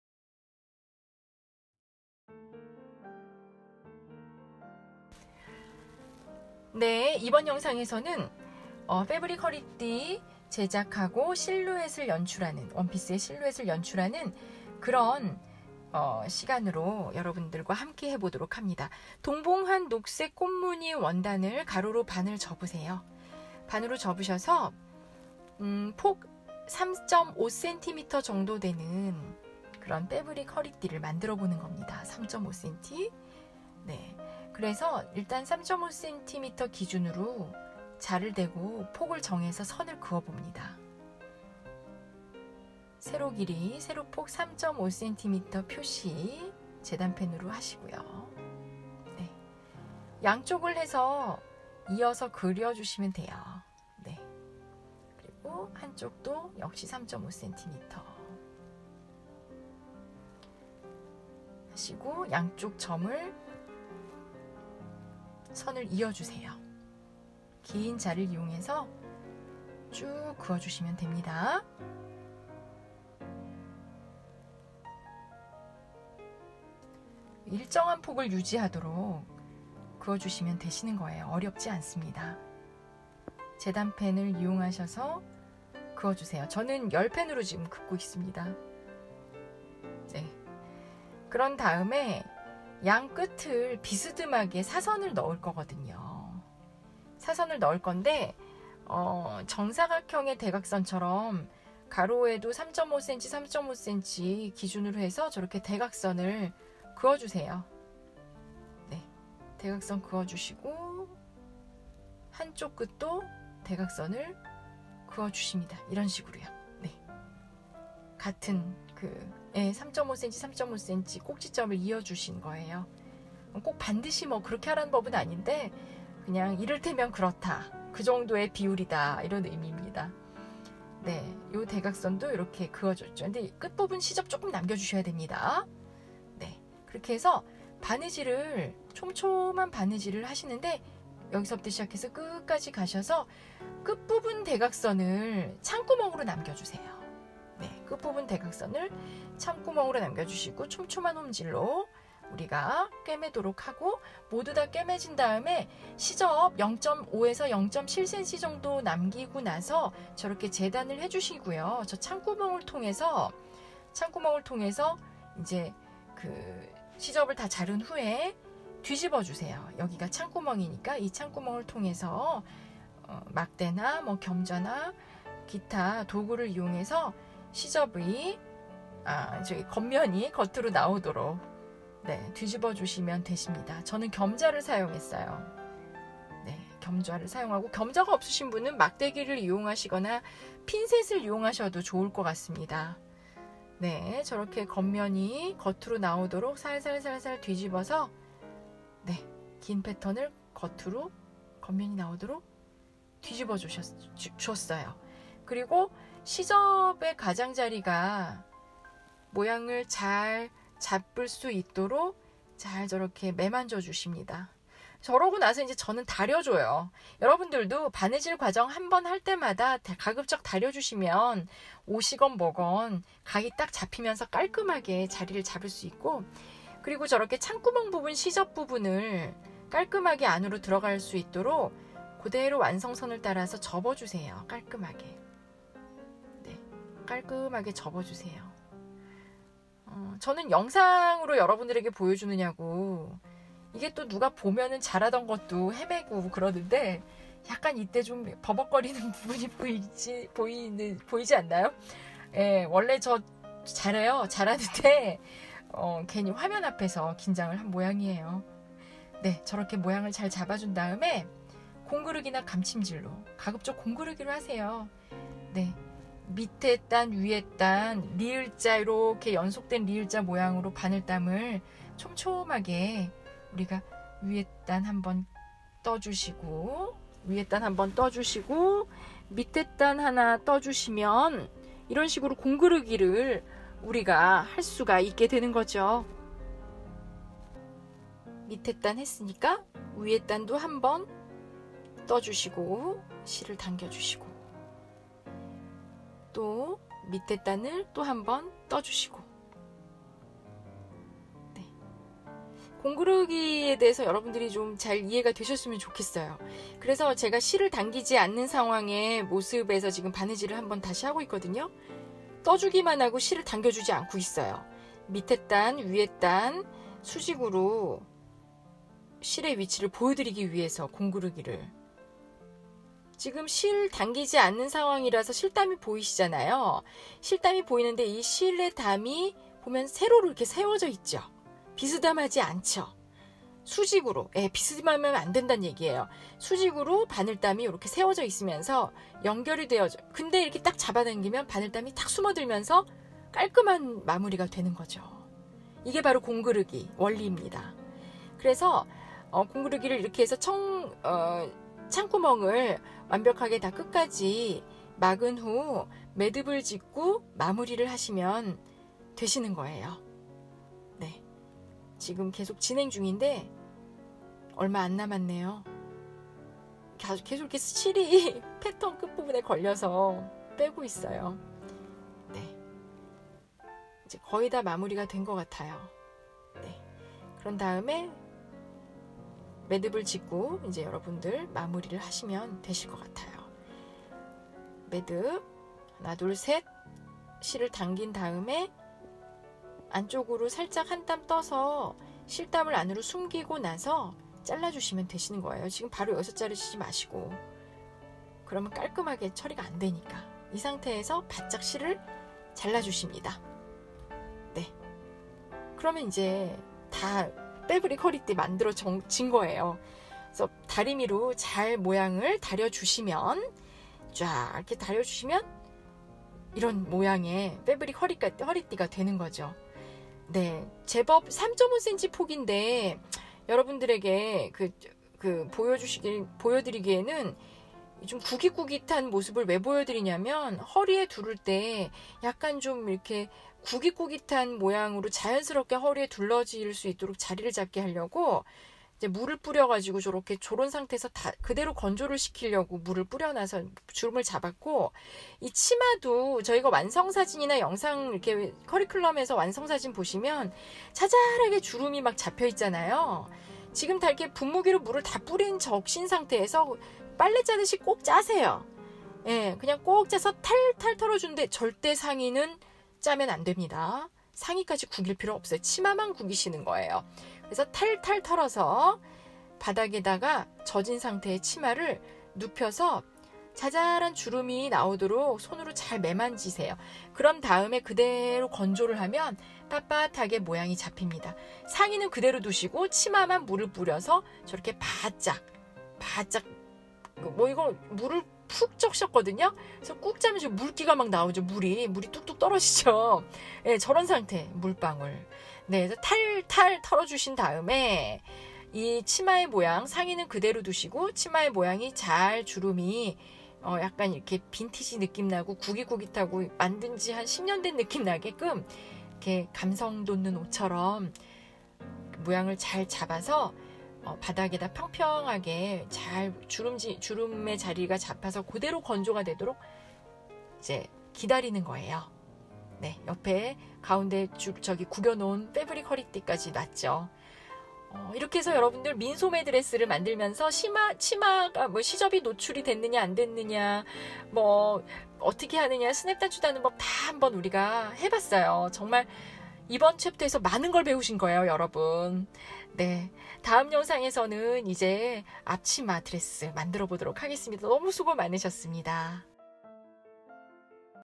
CH4 7민소매드레스의패브릭허리띠제작및실루엣연출
CHAPTER 4-7
[ 민소매 드레스의 패브릭 허리띠 제작 및 실루엣 연출 ]

* 수업 재료 구매를 원하시는 분은 아래의 링크를 참고하세요

(해당 커리큘럼 재료 구매는 옵션에서  시즌1을 선택하시면 됩니다)

[수업목표]
패브릭 허리띠 제작과 드레스 실루엣을 연출하는 간단한 팁을 알아 봅니다.

[준비물]
녹색 꽃무늬 벨트 원단, 바늘, 실, 가위, 뒤집는 도구(겸자, 핀셋, 나무 막대 등)

[수업개요]
긴 패턴을 바느질하여 뒤집고 창구멍을 공그르기 바느질로 마무리하는 과정을 통해 공그르기 기법을 다시금 확인하며 숙지하게 됩니다.

5:29
패브릭 허리띠는 단순히 허리에 두를 수도 있고 머리에 둘러서 헤드밴드로도 활용이 가능하므로 폭이나 길이를 자유롭게 정하여 응용할 수 있습니다.

8:27
주름 모양 잡아 주기

[마지막 정리]
어울리는 원단을 선택하여 패브릭 허리띠를 제작해서 코디를 하여 디자인을 더욱 풍성하게 연출하는 방법을 익힙니다. 폭 넓은 치마의 자잘한 주름을 잡아서 연출하는 방법도 알 수 있었습니다.

[다음 수업 예고]
앞치마와 드레스 set를 만드는 챕터가 시작됩니다.